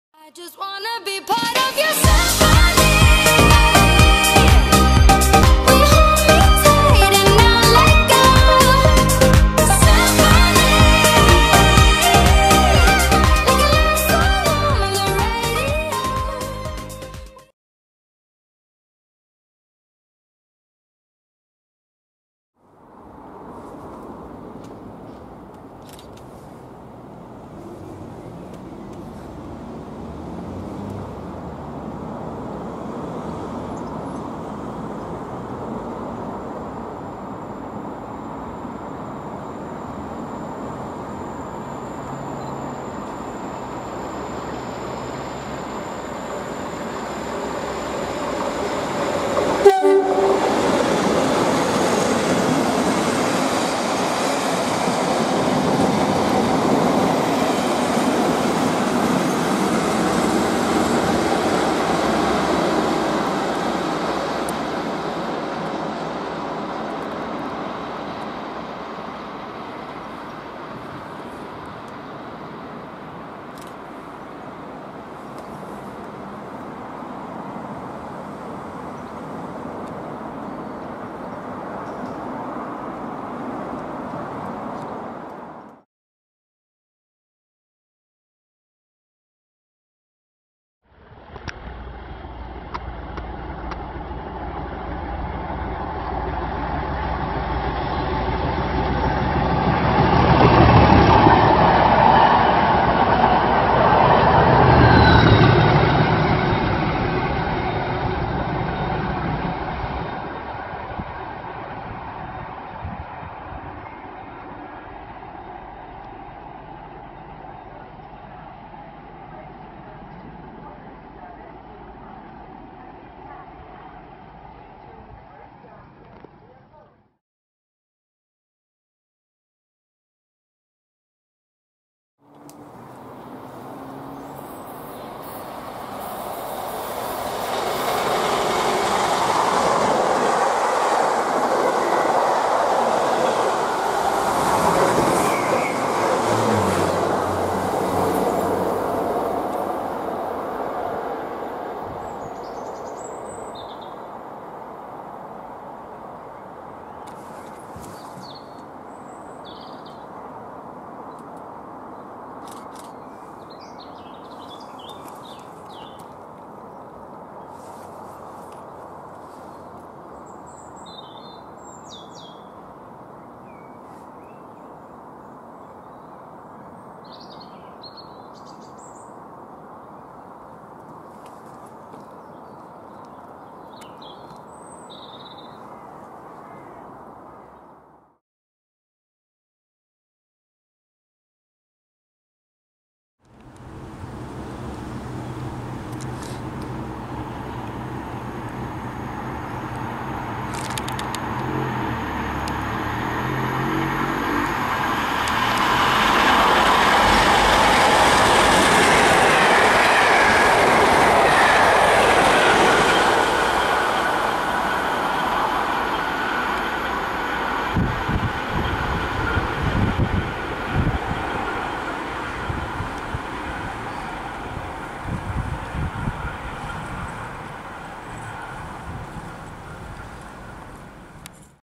(HD) Colas 37025 & 37057 @ CHU,HDG,GTY,URM (10/05/17) and (11/05/17)
In this video we see 37025 and 37057 a total of 4 times in 2 days! We see it idle through Cheadle Hulme with a 2 tone. Then we see the beasts thrashing through Heald Green and then 1 stop down at Gatley we see them idle through with close bowlage. Finally we see them idle through Urmston. We hope you enjoy!

Train information for the (10/05/17) :
1Q43 18:38 CREWE C.S. (L&NWR SITE) - 07:31 CREWE C.S. (L&NWR SITE)
Runs on 10/05/2017
VSTP schedule runs WO from 10/05/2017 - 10/05/2017
Operated by Colas IM
Test Trains
Diesel Locomotive
Timed to run at 168mph
10/05/2017 18:32
Train Activated
Train ID - 42149W2X10 (Masked ID 149W)
10/05/2017 23:58
UNSCHEDULED report received at HYDE JN
11/05/2017 00:16
UNSCHEDULED report received at HADFIELD

Train information for (11/05/17):
1Q44 18:38 CREWE C.S. (L&NWR SITE) - 07:31 CREWE C.S. (L&NWR SITE)
Runs on 11/05/2017
WTT schedule runs ThO from 15/12/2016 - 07/12/2017
Test Trains
Diesel Locomotive
Timed to run at 75mph
First & Standard Seating
11/05/2017 13:13
Train Activated
Train ID - 421Q44CX11 (Masked ID 165W)